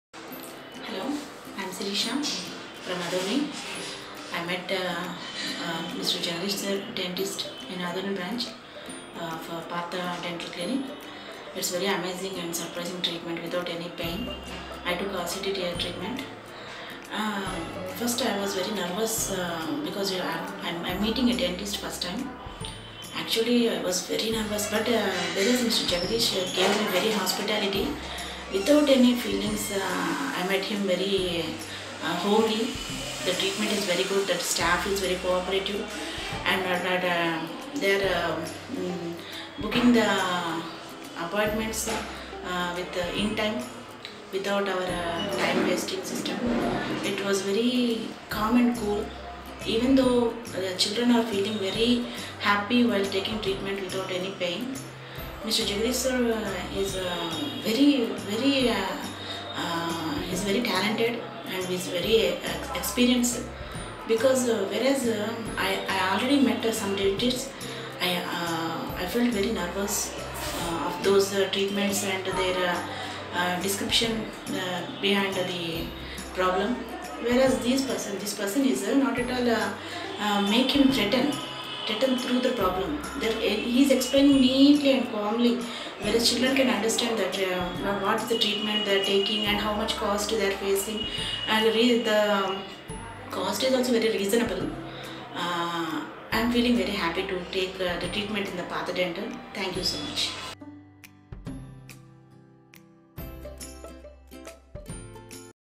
happy patient testimonial after Root Canal treatment II Partha Dental II
Quick actualities on torment after a root waterway: A root trench will treat the ailing tissue (mash) while protecting whatever is left of the tooth. An individual will be given soporific before the methodology, so it is generally not any more excruciating than a run of the mill dental filling. On the off chance that a root channel falls flat, re-trying it can fix the issue